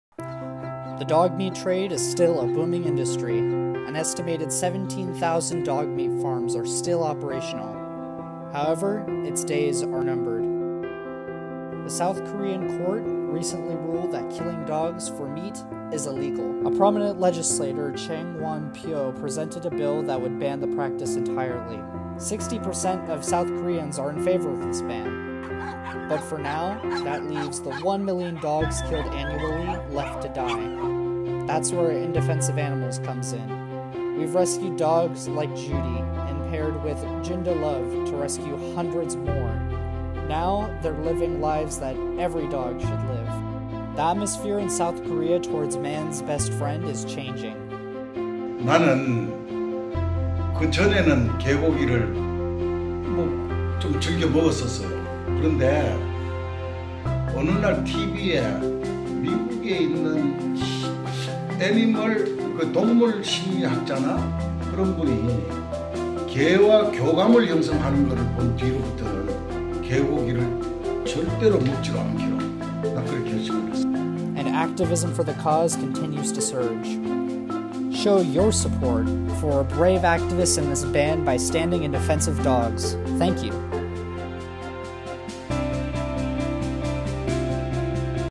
Time is Running Out for the Dog Meat Trade!
In Defense of Animals is going the distance in our fight against the dog meat trade. The best part? We. Are. Winning! Watch this video to find out how your support for our Ditch Dog Meat campaign has allowed us to make real strides against this outdated and barbaric tradition.